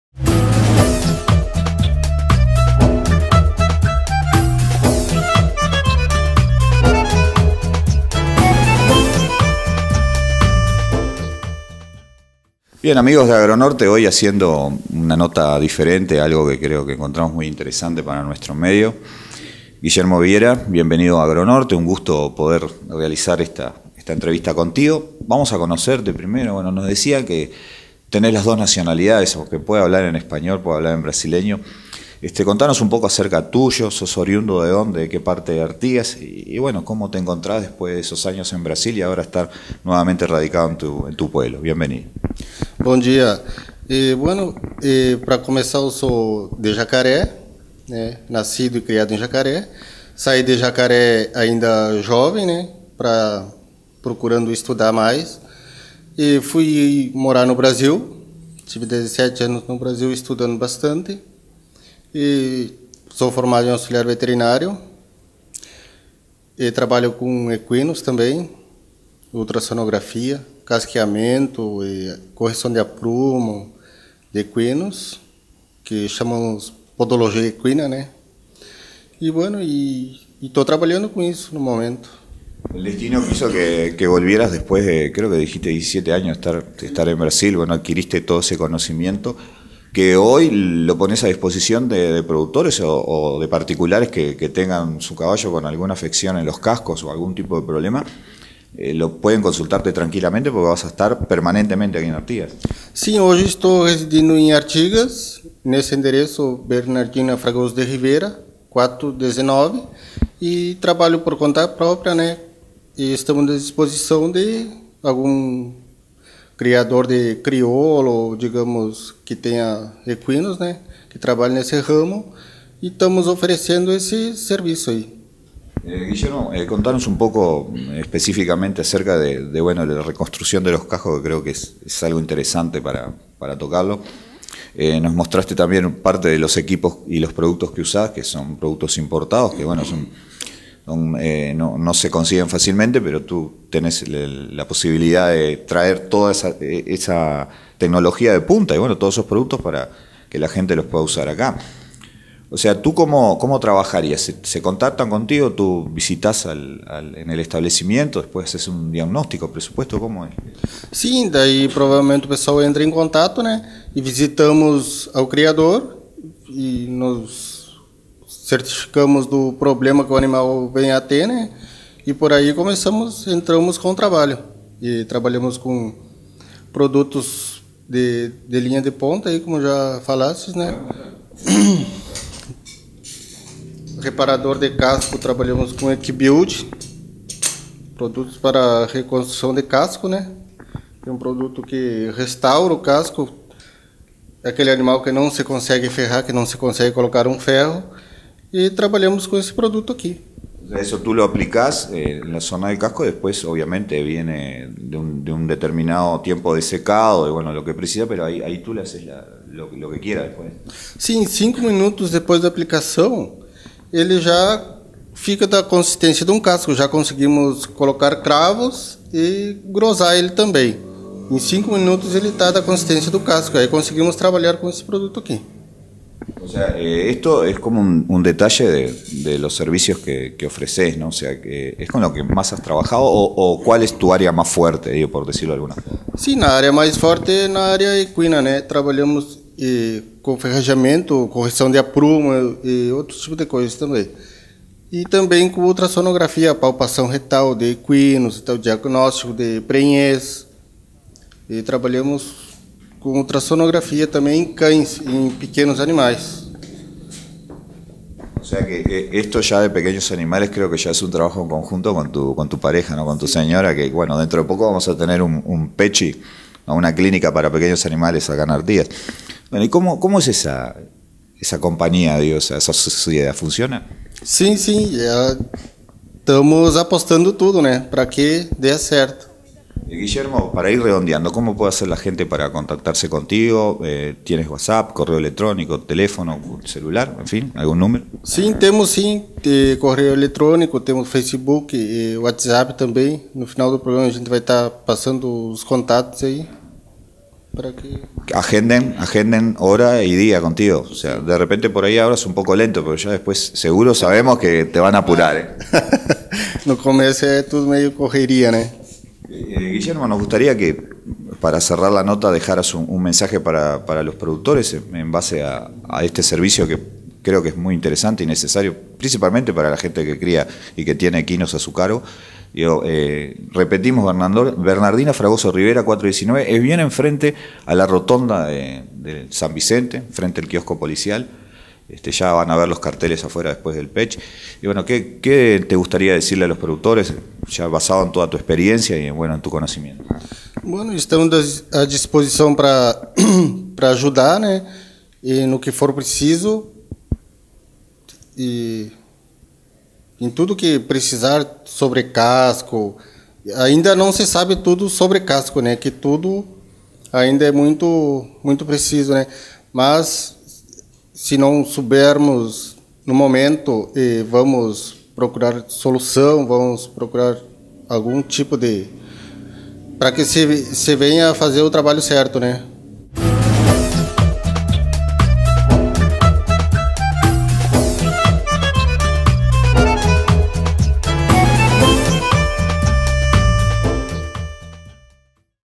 AGRO 5 - AGRONORTETv - ADELANTOS TECNICAS EN PODOLOGIA EQUINA
ENTREVISTA CON GUILLERMO VIERA